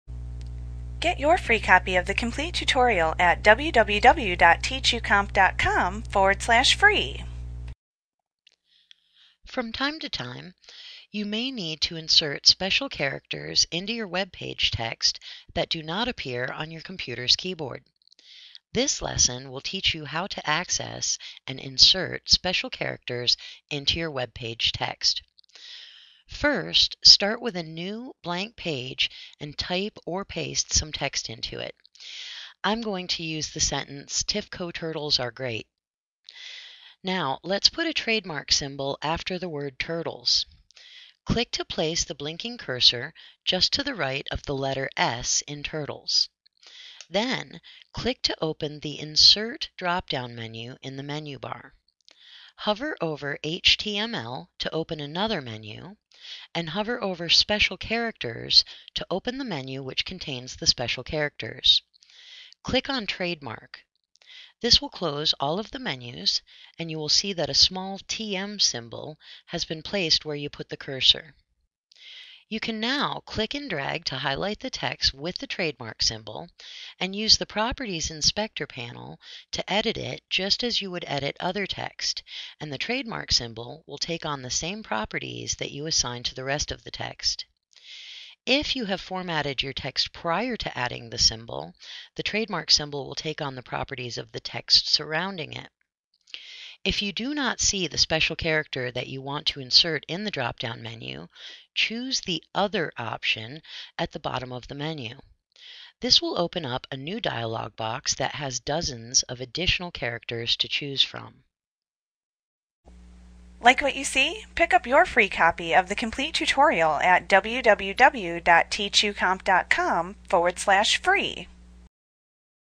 Dreamweaver CS5 Tutorial Special Characters Adobe Training Lesson 3.11
Learn about special characters in Adobe Dreamweaver A clip from Mastering Dreamweaver Made Easy v. CS5. - the most comprehensive Dreamweaver tutorial available. Visit us today!